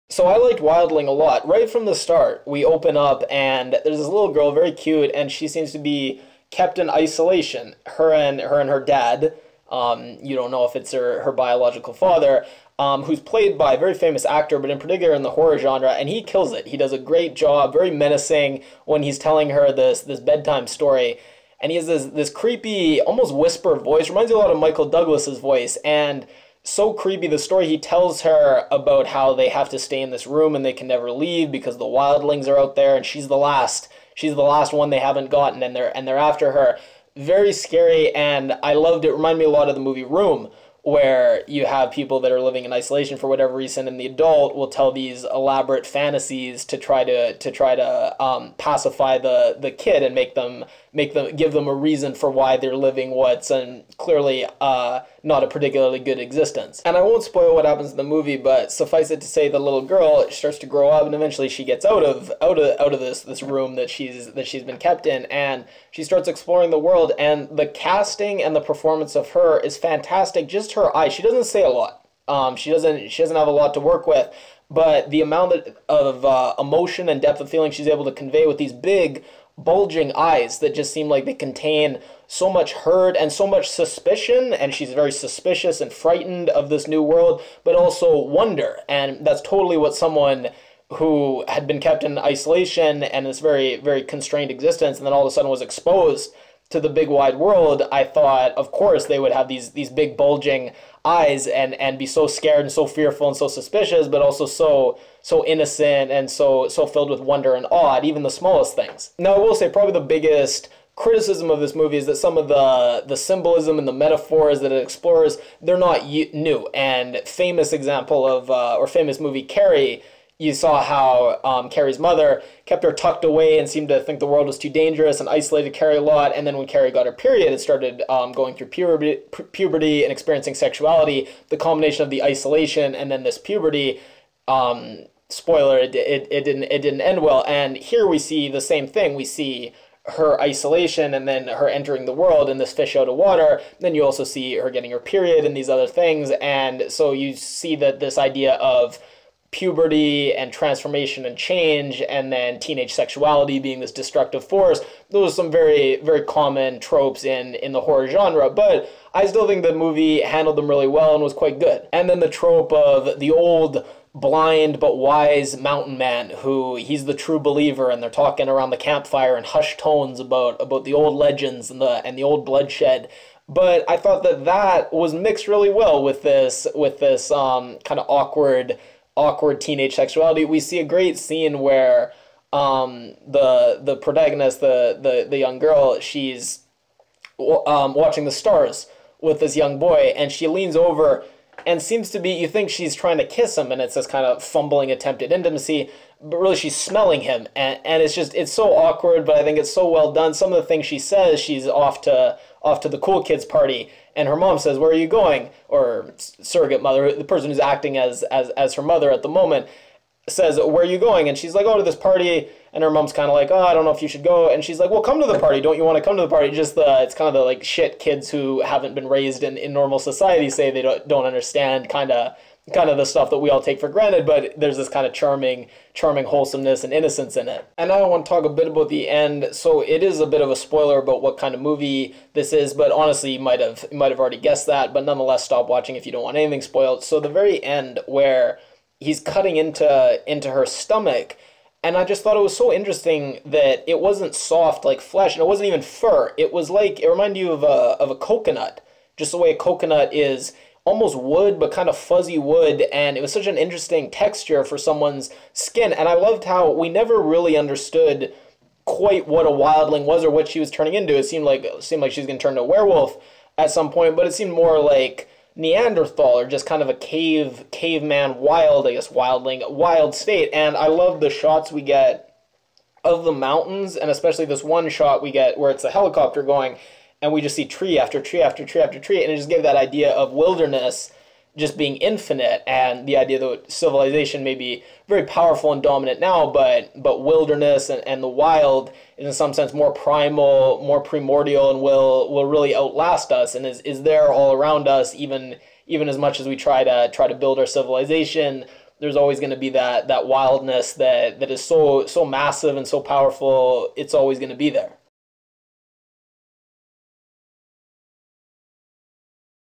Wildling (2018) Movie Review
All about teenage sexuality and adolescence and puberty being akin to transformation into a werwolf - all hormones and hair. Wildling is a 2018 American horror-fantasy film directed by Fritz Böhm starring Bel Powley, Brad Dourif, Liv Tyler and Collin Kelly-Sordelet, and co-starring James LeGros, Mike Faist and Troy Ruptash. The screenplay was co-written by Böhm and Florian Eder. The plot follows Anna, a blossoming teenager who uncovers the dark secret behind her traumatic childhood. On video on demand in 2018.